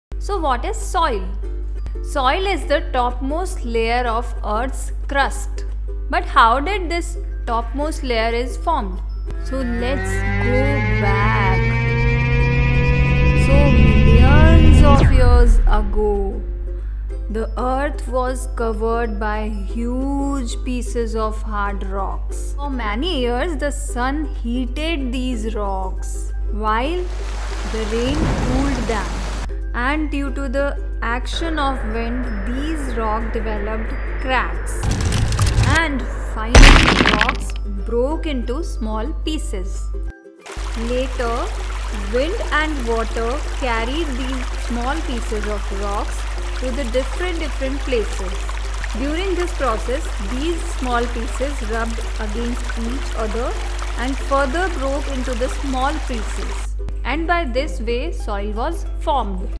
Soil and Its Types | MAJOR SOIL TYPES IN INDIA | Class 10
In this video, we're going to talk about soil types in India. We'll cover the different types of soil in India and how to identify them. We'll also describe the different uses for each type of soil, and give a full explanation of how classification works.

If you're interested in learning more about soil types in India, then this is the video for you! By the end of this video, you'll have a better understanding of the different types of soil in India and the ways in which they're used.